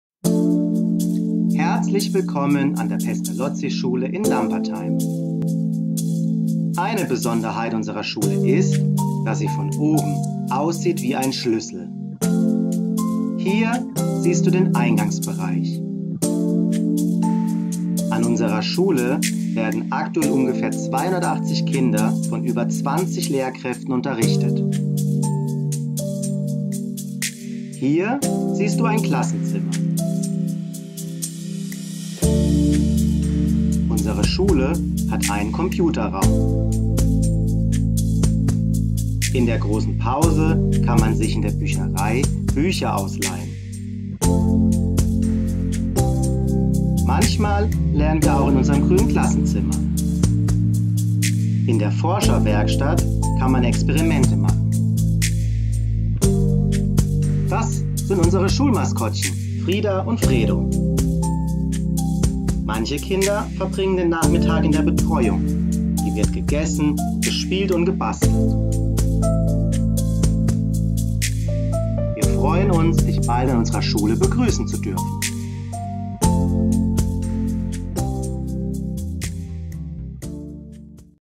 Ein virtueller Rundgang durch die PESTALOZZISCHULE in Lampertheim
Die Pestalozzischule in Lampertheim stellt sich vor...

Aufnahmen, Schnitt und Sprecher: Christian Plenert